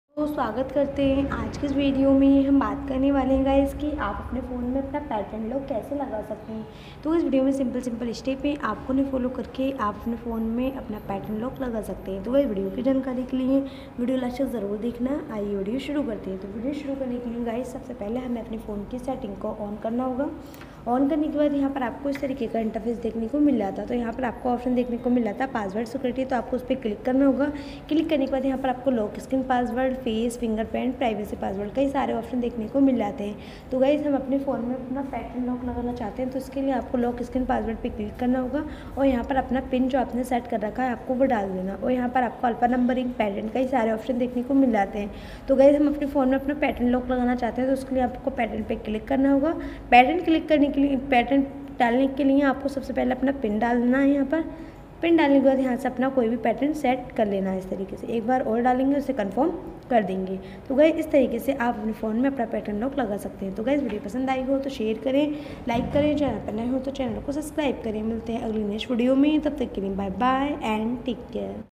How To Set Screen Lock Pattern In Oppo A54 5G, Oppo A54 5G,Screen Lock Password Setting
How_To_SetscreenlockPassword InOppo A54 5G,